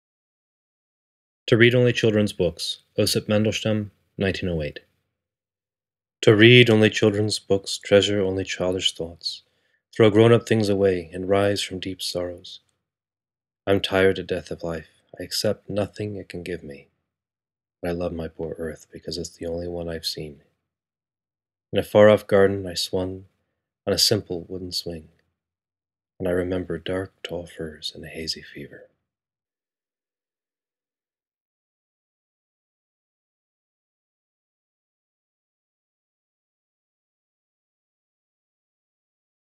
Mandelshtam—To read only children's books
A reading of Osip Mandelshtam's 1908 poem, "To read only children's books."